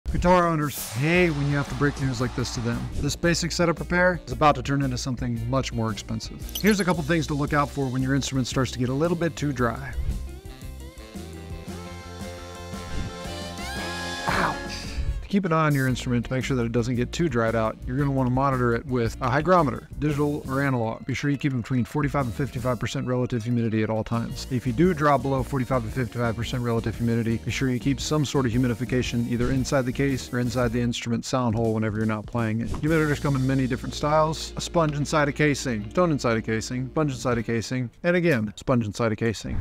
Keep your Guitar Hydrated to Prevent This
Dive into the art of cleaning and restoring your guitar's essential parts. Let's bring the sparkle back to your music! 🎶🔧